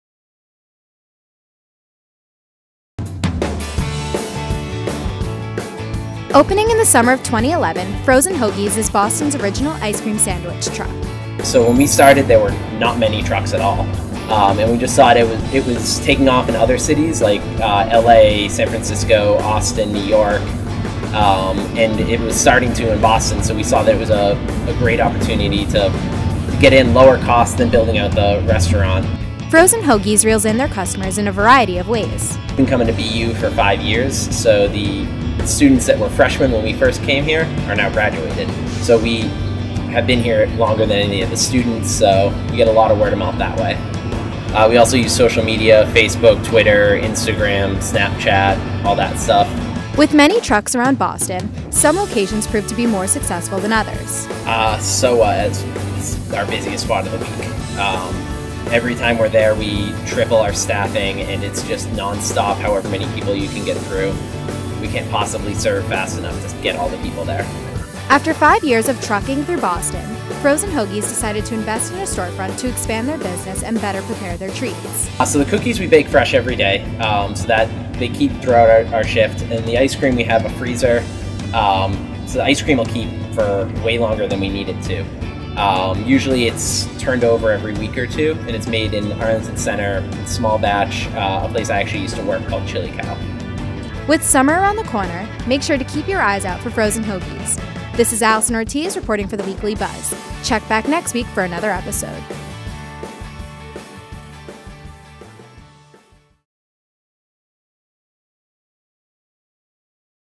Frozen Hoagies Food Truck
The Weekly Buzz's Alison Ortiz finds out what makes this Boston food truck so successful.